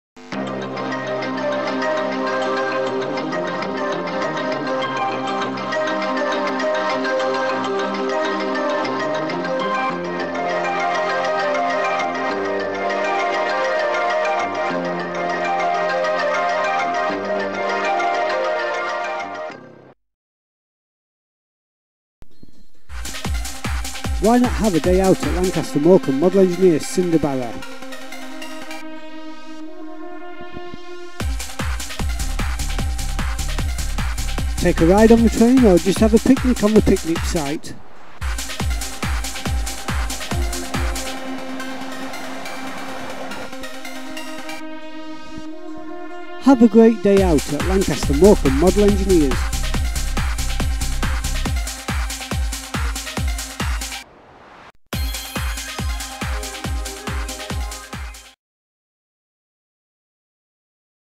LMMES ADVERT wmv
A little advert I made for Lancaster and Morecambe model engineers society  Cinderbarrow picnic site  Yealand  near Carnforth  LA5 9RX on your sat  nav